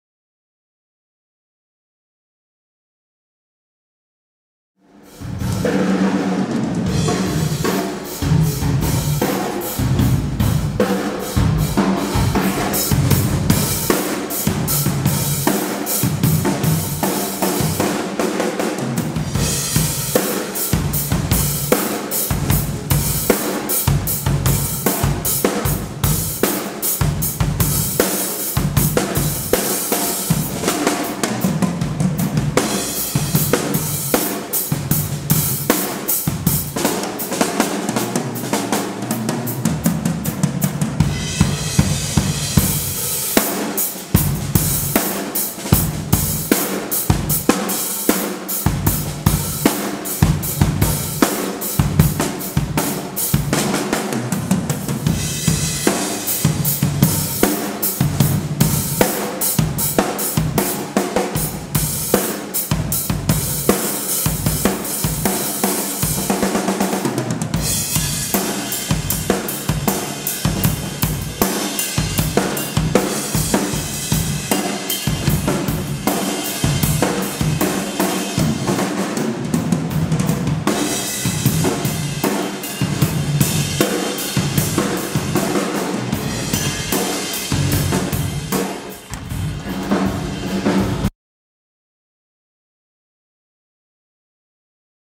The Fields of Near & Far : Dunnett Titanium
Cole George playing a Dunnett power Titanium kit. 14 / 16 / 18 / 26. Audio is direct from the camera mic, a Canon 5D MKIII.  Shot at the Warehouse Studios in Vancouver (Thanks Brian!)